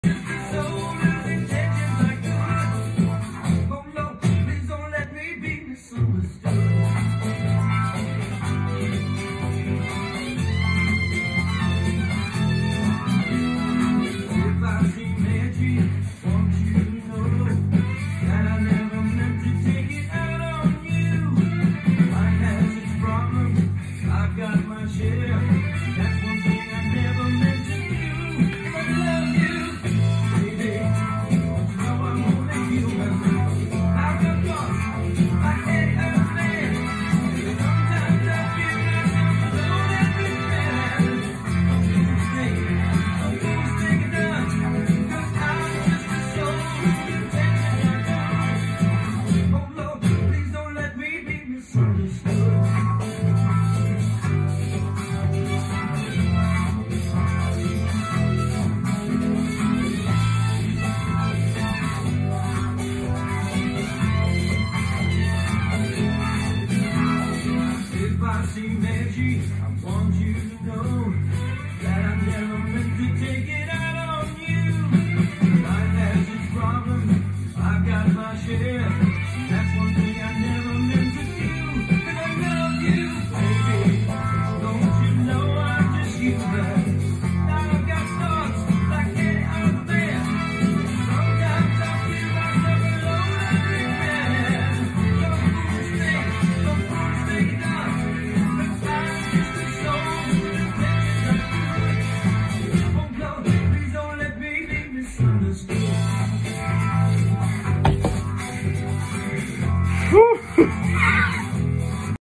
Dance! first strike against paralysis-by-fear of covid-virus...
Dance! +generate a fighting-form fever to fight back first strike against paralysis-by-fear of covid-virus...
+fun free-form dancing to get moving to this power inside music to lift spirits; add one healthy distraction, free and easy, joyous laughter giggles included, no batteries necessary...